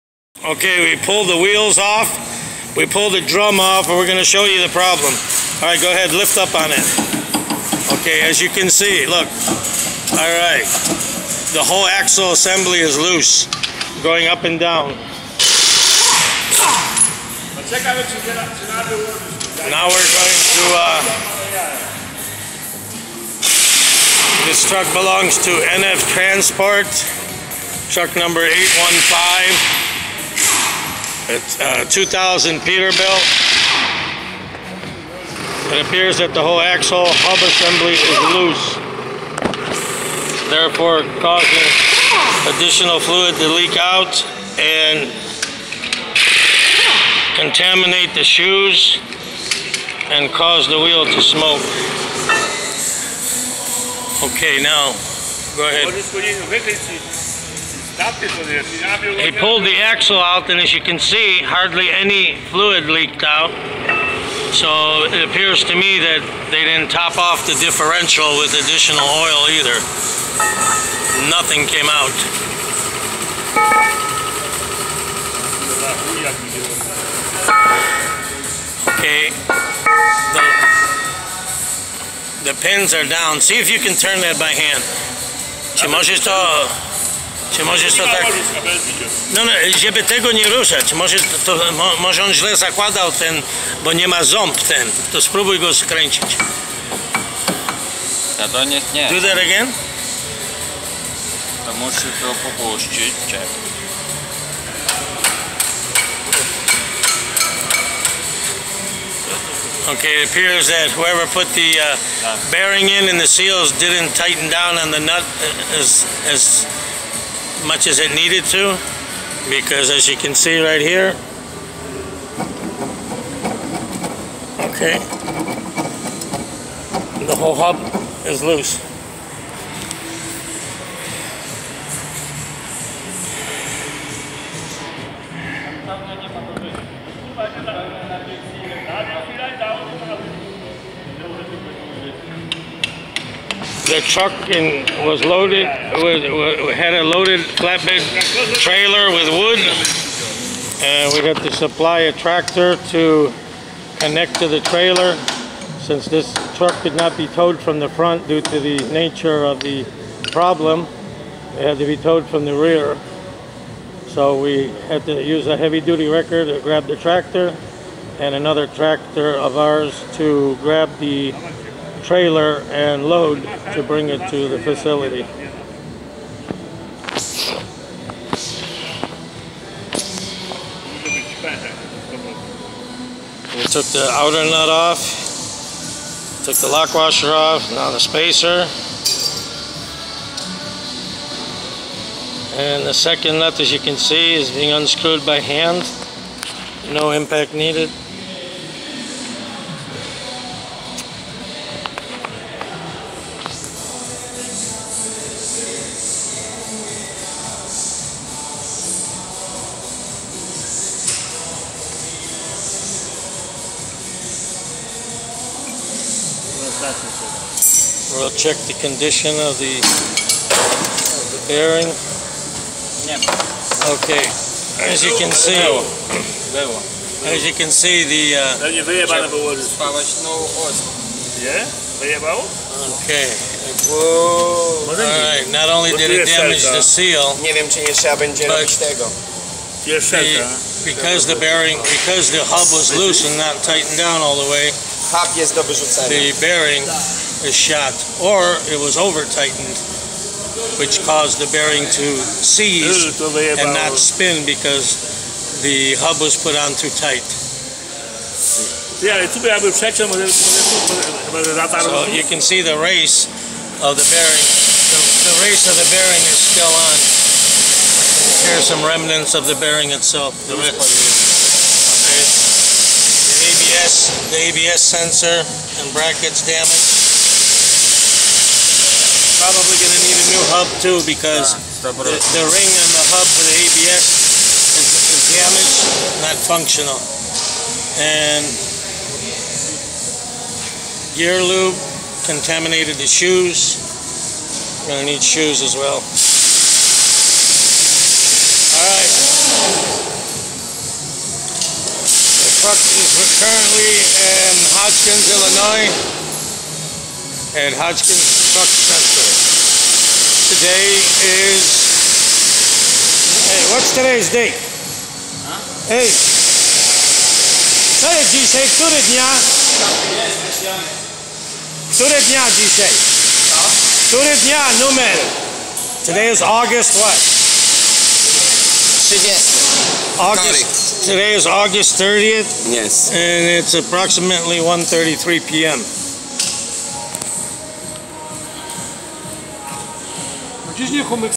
What not to do when replacing semi wheel hub seals
Broken bearing after seal replacement. Reason for damage was over tightening  of hub retainer nuts causing bearing to not spin and sieze up.